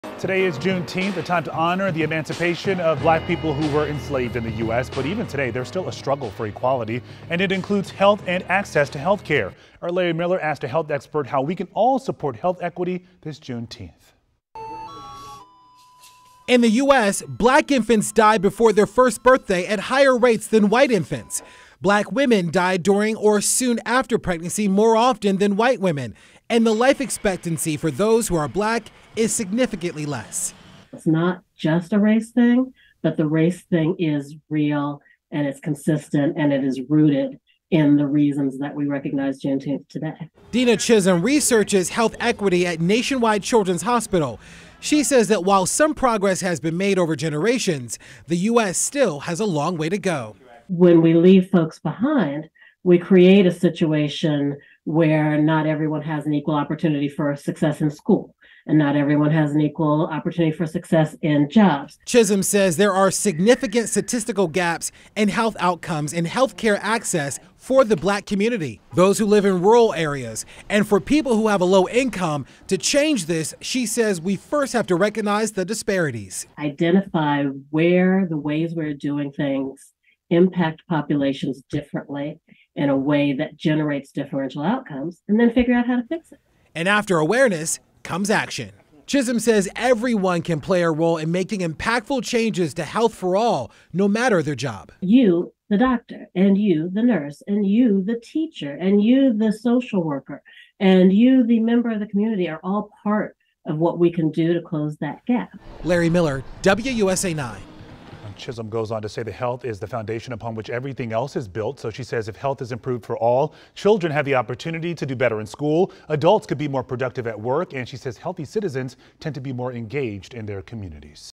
Supporting health equity on Juneteenth
Larry Miller asked a health expert how we can all support health equity this Juneteenth.